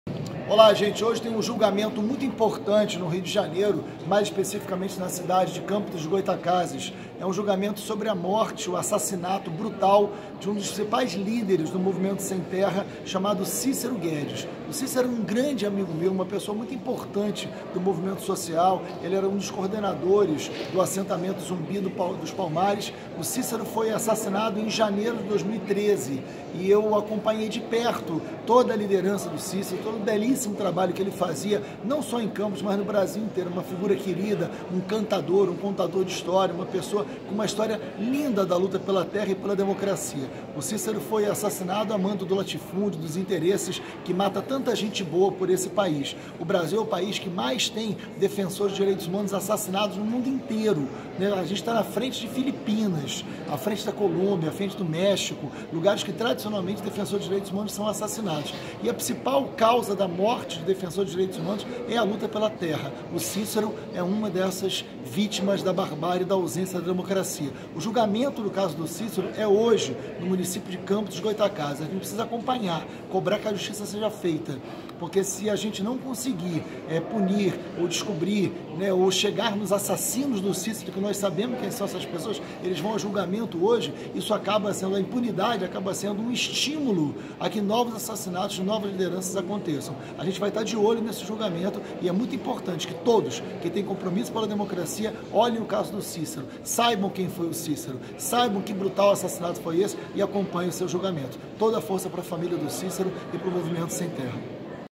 Justiça para Cícero Guedes, líder do MST assassinado em Campos
O suspeito de ter assassinado o líder  do MST Cícero Guedes em janeiro de 2013, na cidade de Campos dos Goytacazes, será levado a júri popular nesta quinta-feira (07/11). Exigimos justiça a Cícero e o fim da violência contra os trabalhadores sem terras.